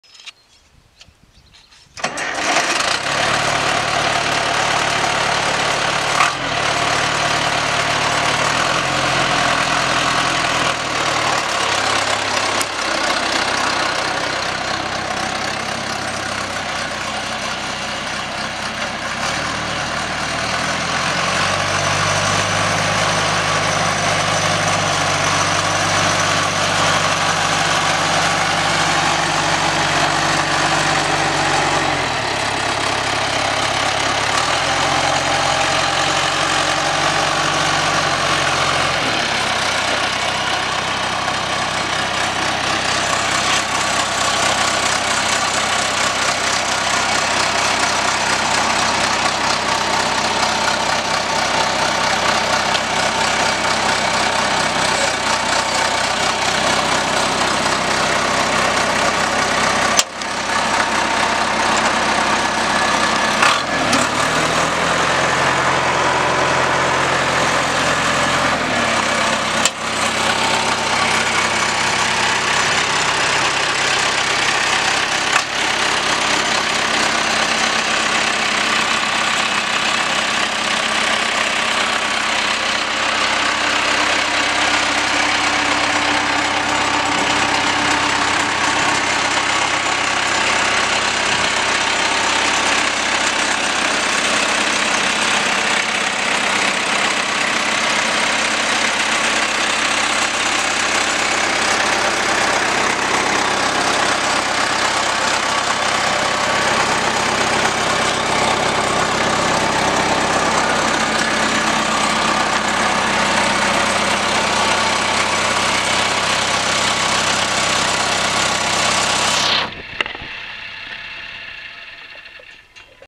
1990 John Deere 2640 2WD Tractor Selling June 3, 2020 on Big Iron Online Auction